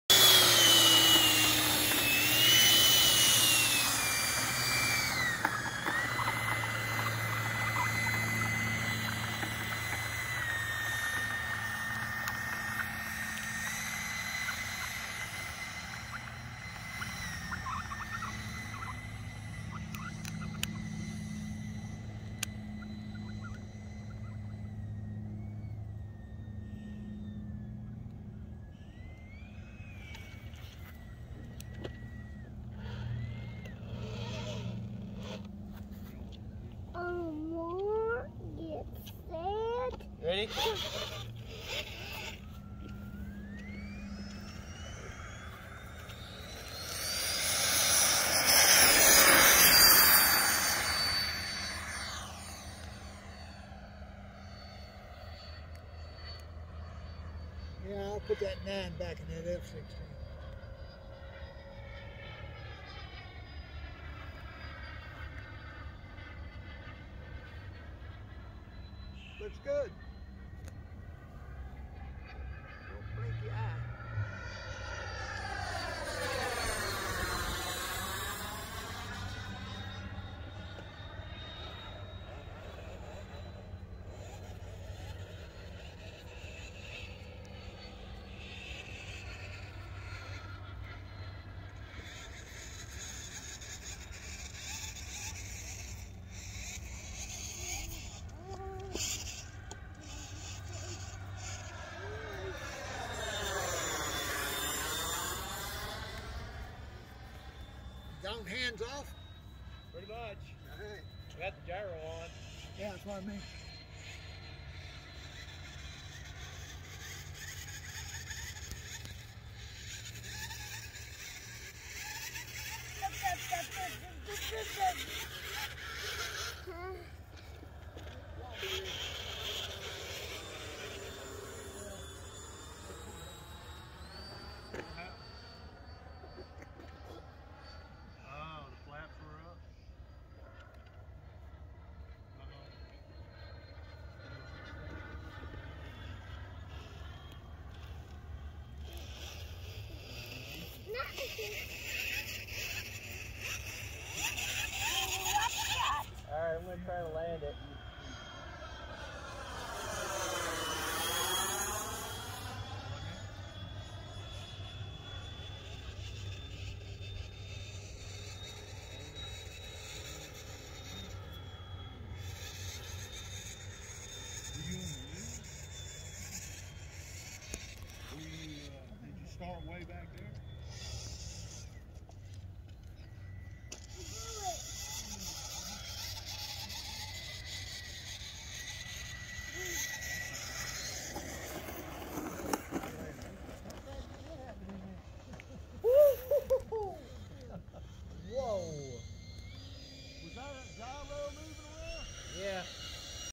Freewing F-104 Starfighter 90mm First Flight
First flight of my second Freewing F-104 Starfighter 90mm.  Replace the stock gyro with a Futaba GYA-553 and flew with the CG set to 75mm.